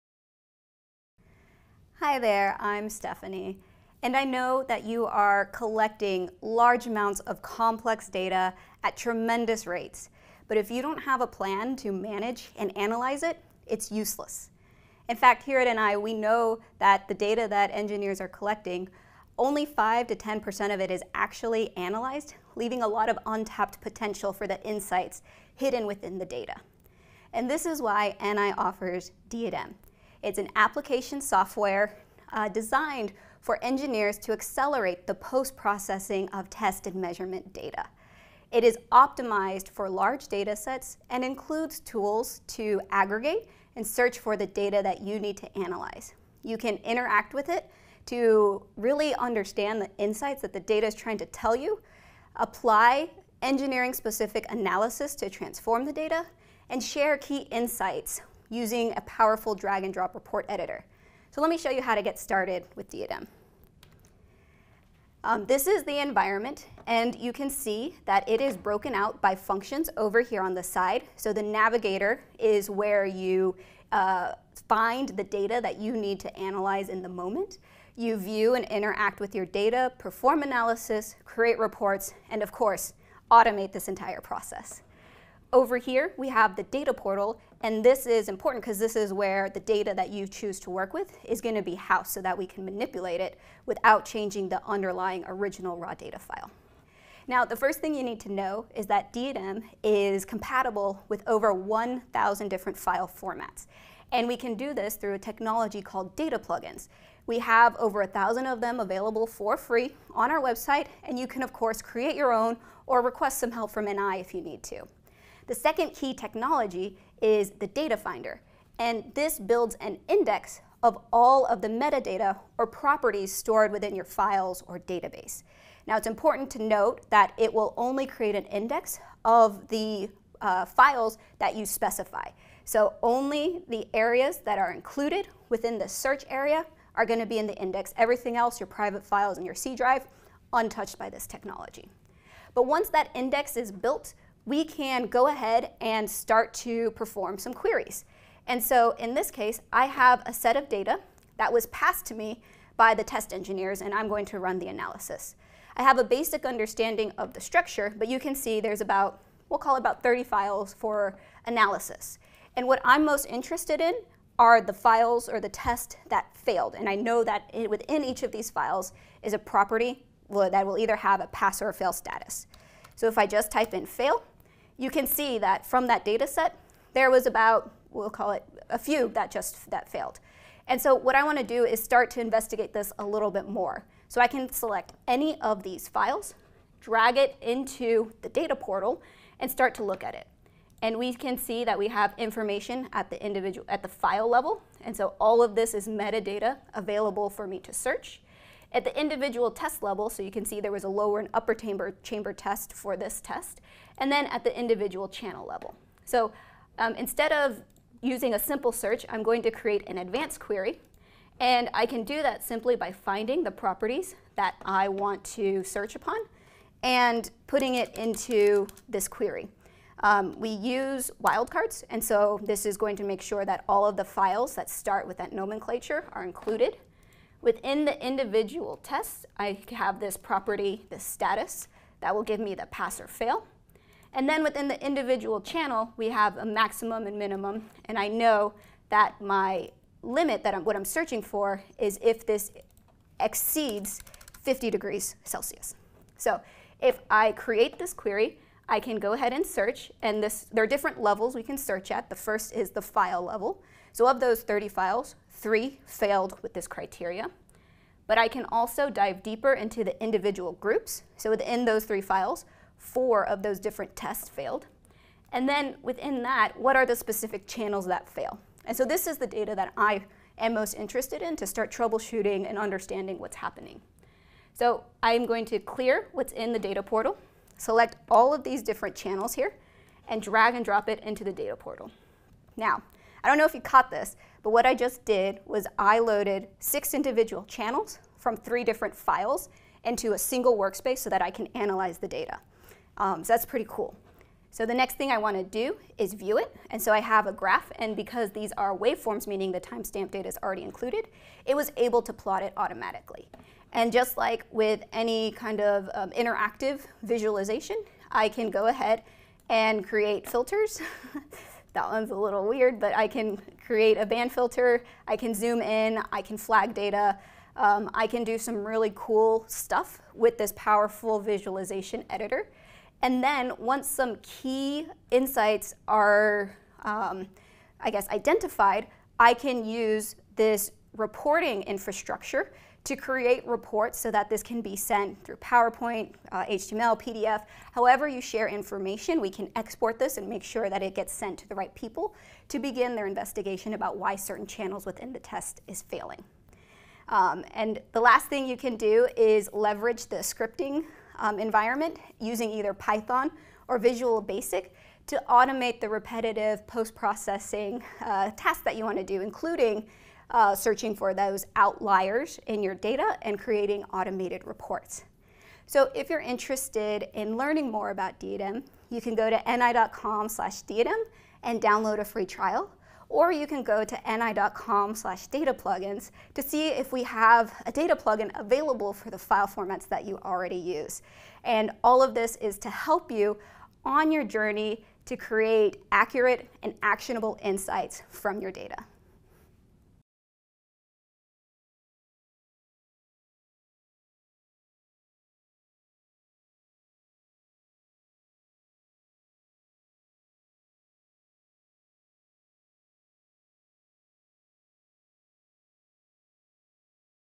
What is DIAdem?
Has Your Engineering Data Outgrown Excel? Optimize your data analysis through DIAdem by accessing any data file or type, leveraging built-in visualization and analysis, creating custom reports, and automating tasks for efficiency.